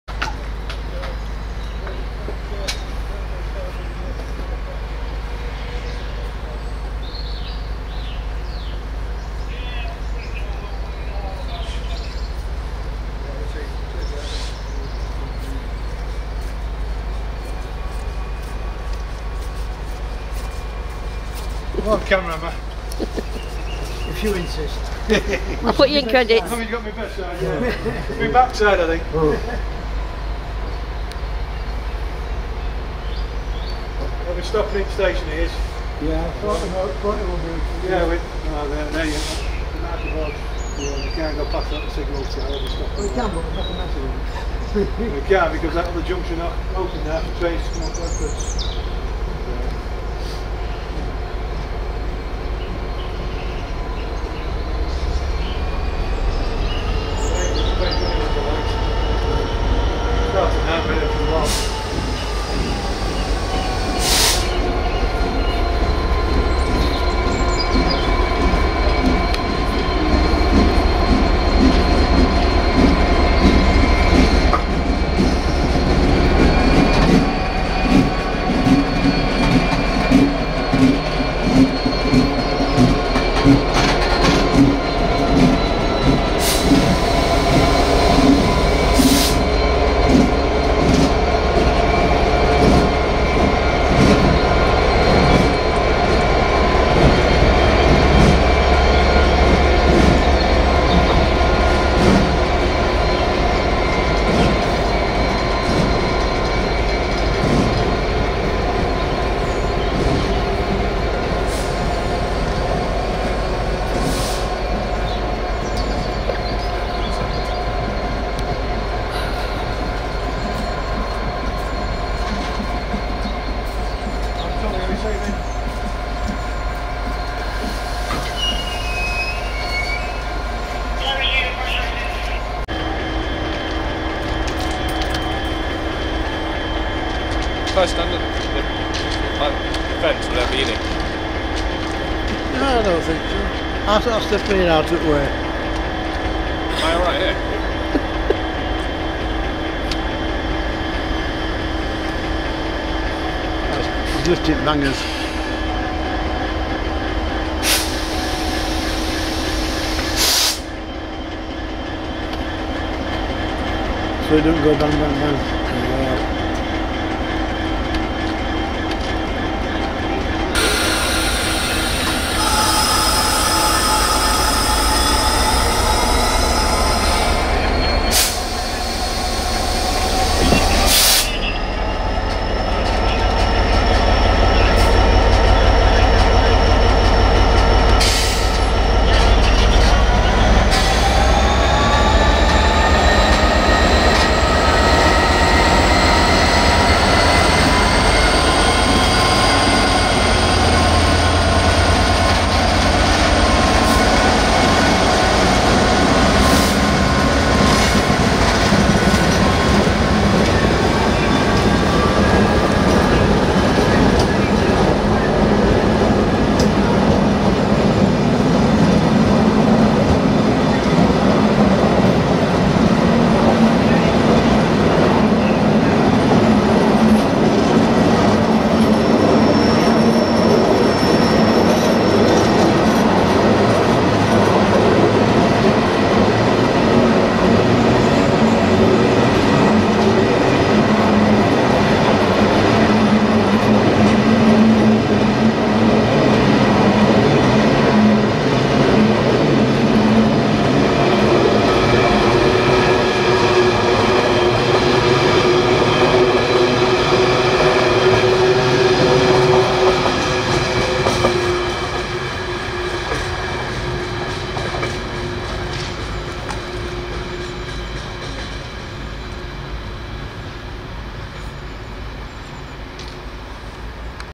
66604 at Shipley on 02/02/2020
6Y30 1135 Shipley to Belmont Down Yard
02/02/20 at Shipley
66604
Departing at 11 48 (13m late)
This movement is carrying out engineering works